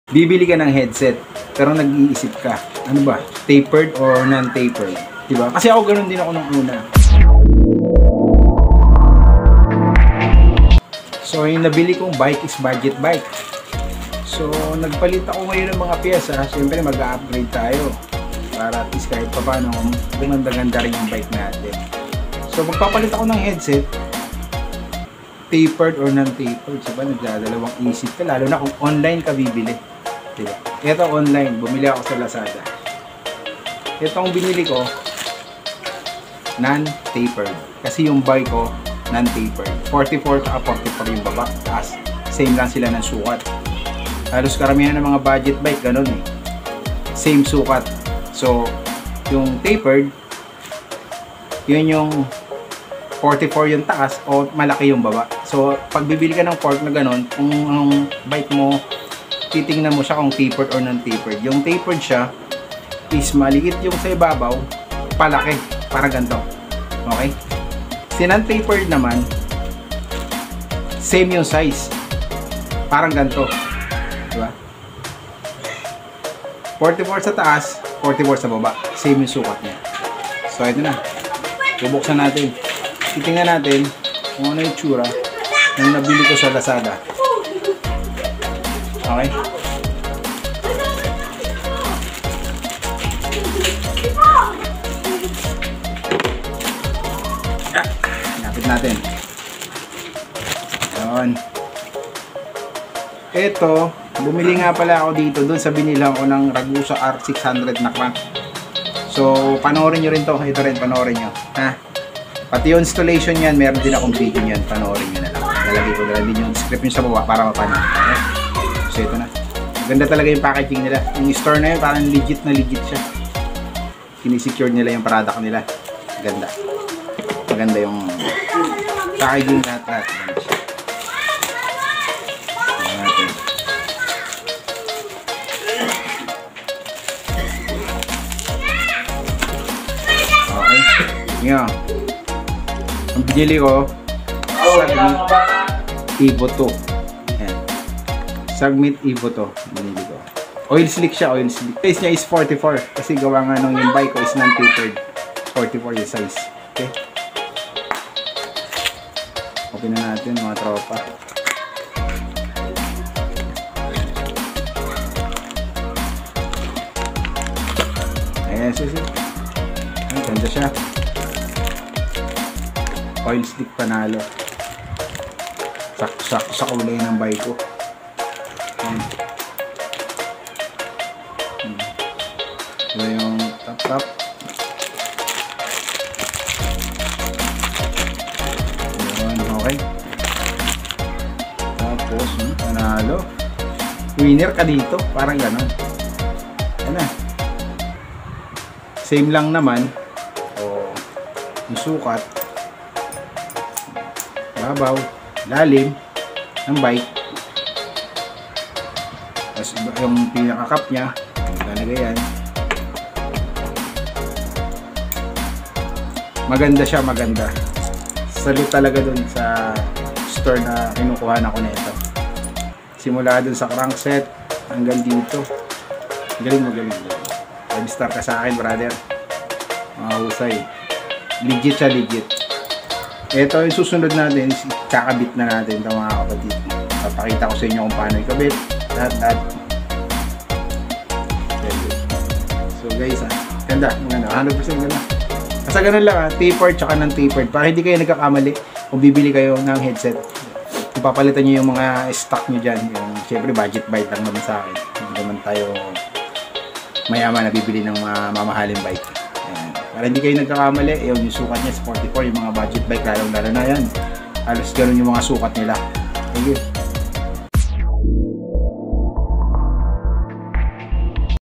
Sagmit Evo2 Non Tapered from lazada sulit nga ba?|Unboxing|MTB headset
Unboxing evo2 headset from Lazada

Sa halagang 355 pesos only mga ka Halo-Halo